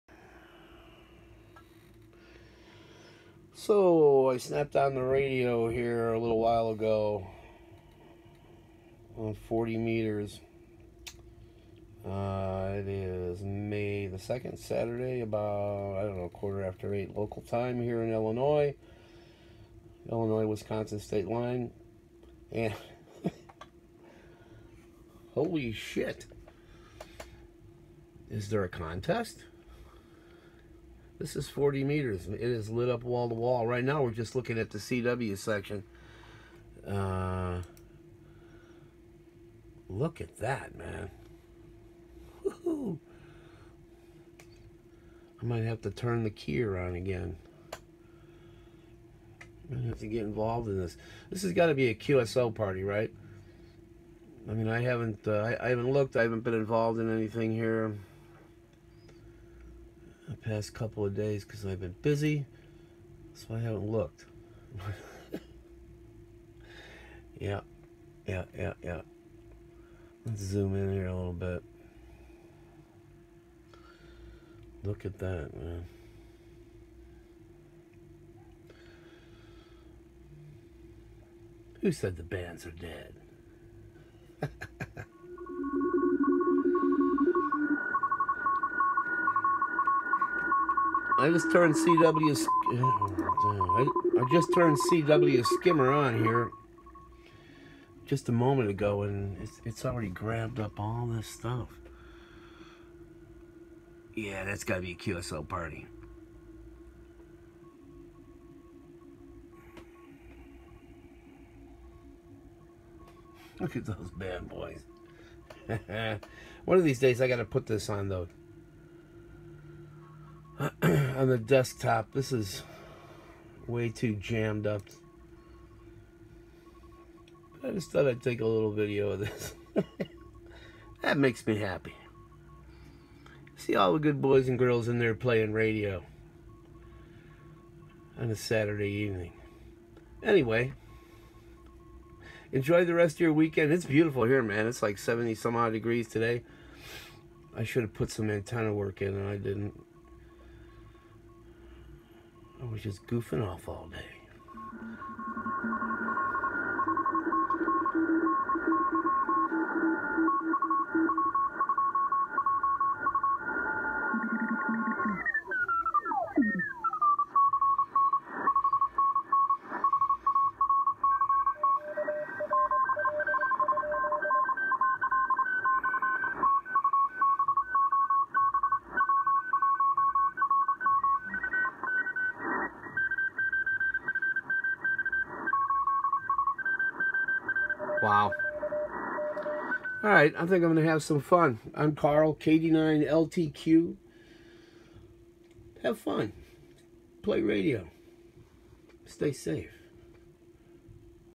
SDR Play RSP1A as a Panadapter on my Kenwood TS-590SG.
SDR Play RSP1A as a Panadapter on my Kenwood TS-590SG.CW Skimmer thrown in for good measure.  Busy night.